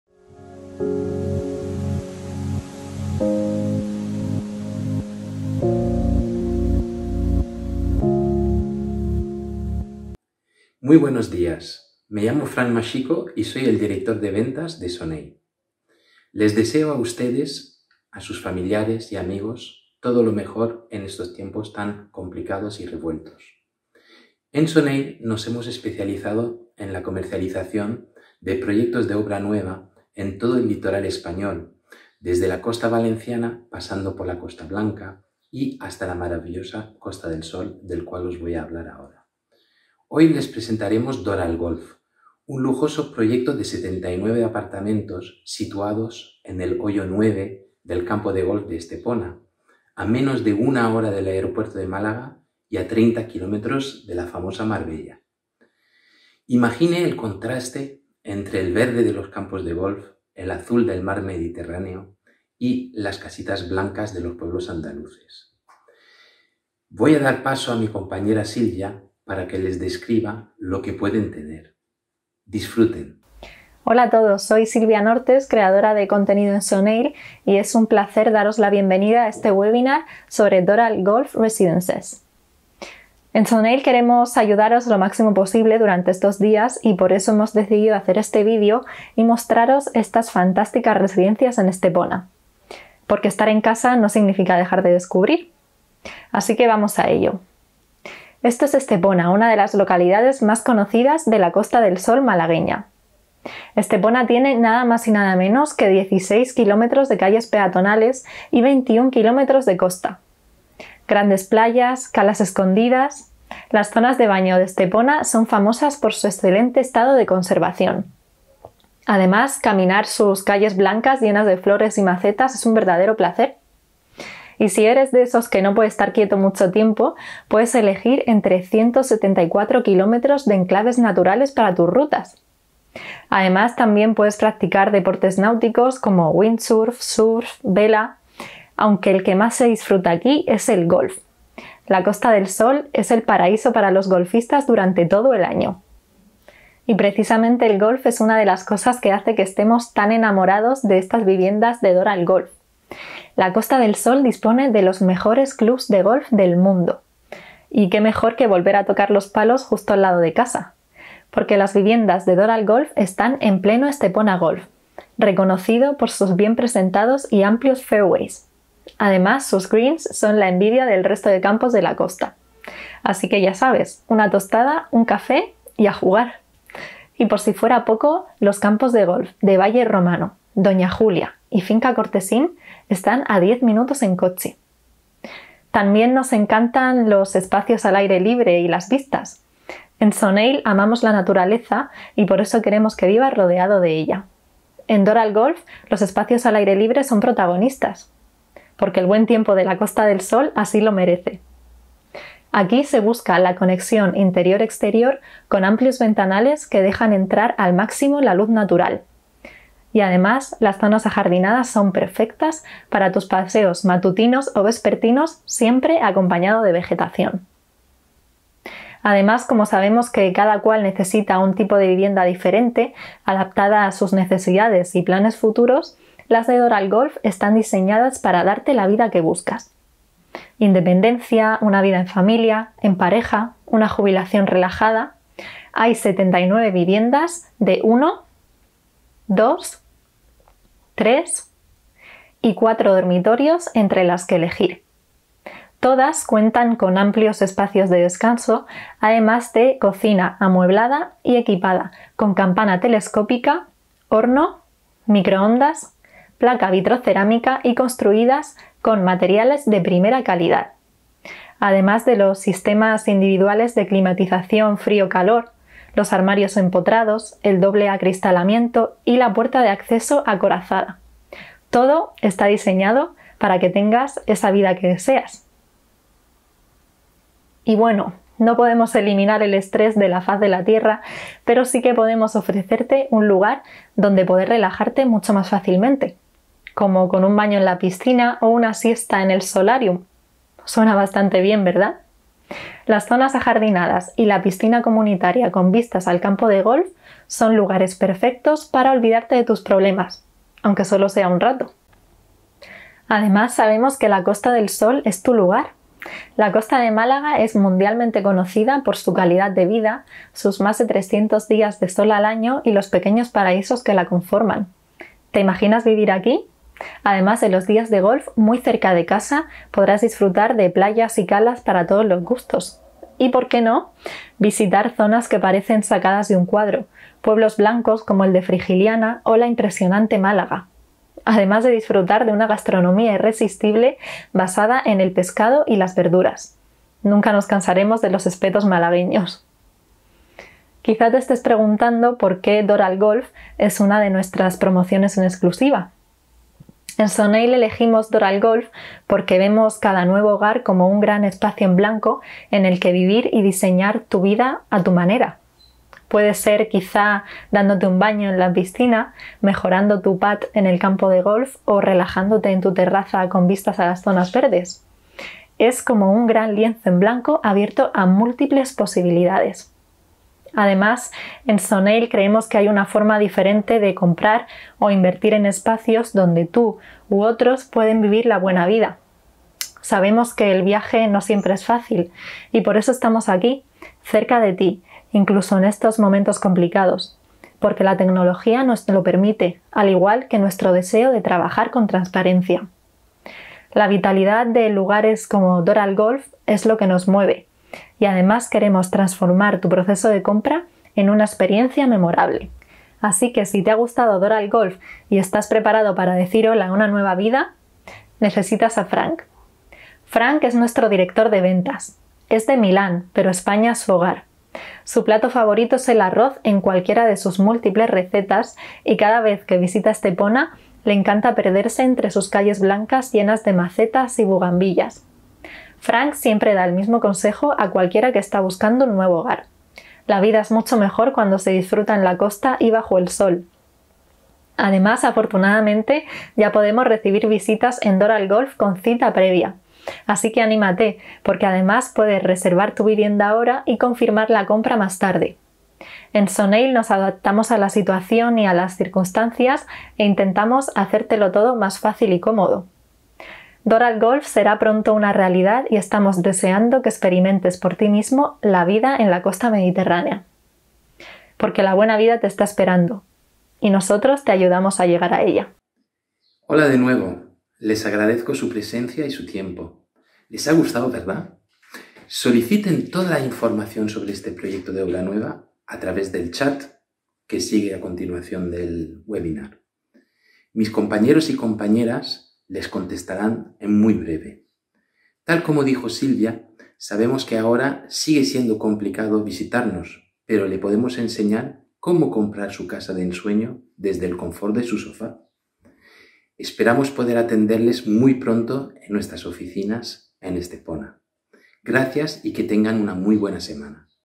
Doral Golf Residences desde tu sofá
Un webinar para que descubras Doral Golf Residences, nuestra promoción exclusiva en el campo Estepona Golf.

¡Descubre este espectacular residencial ubicado en una de las mejores zonas de la Costa del Sol desde la comodidad de tu sofá!